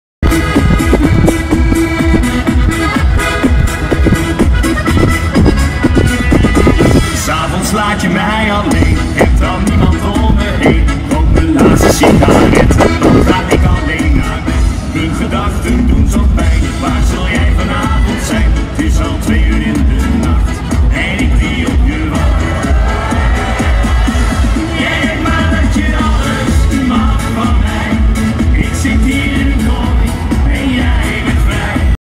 Carnaval 2011 Barger Oosterveld opstellen. Dörzetters '98 Weiteveen
Opstellen in Barger Oosterveld Carnaval 2011   Dörzetters '98 Weiteveen